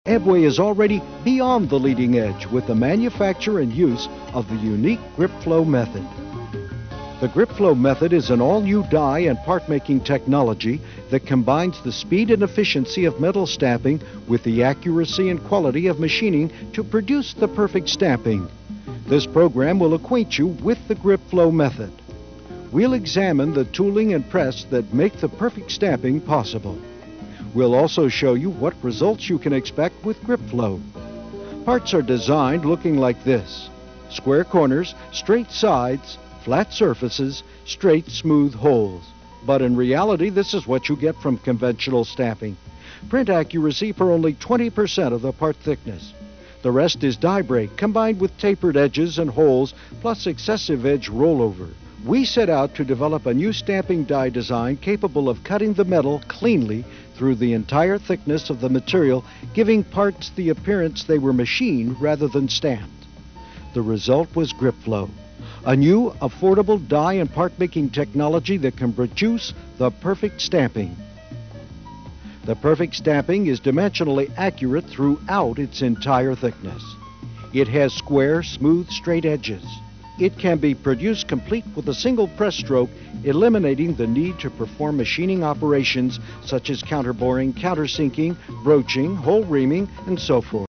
GRIPflow Metal Stamping Process
Explains the Gripflow Metal Stamping Process and shows parts being made in a Gripflow Press.